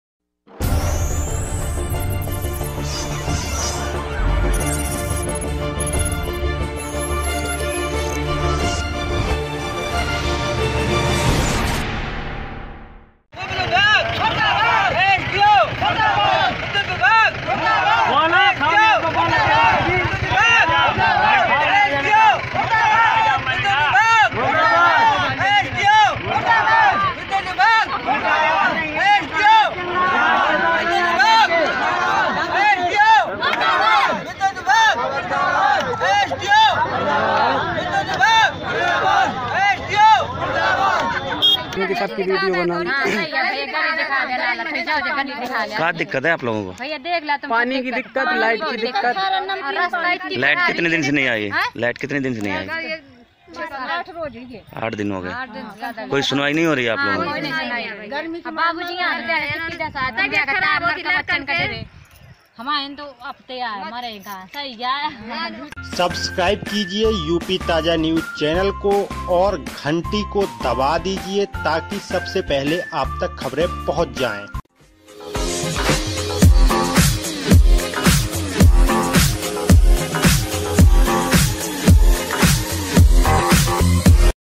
मौदहा में बिजली न आने पर मुहल्लेवासियों ने किया राजमार्ग जाम
संपादक
गनपत सिंह महान

TAJA NEWS
TAAZA NEWS
TAJA Khabar
taja khabar
taja news
hindi news
bundeli News
Bundelkhand news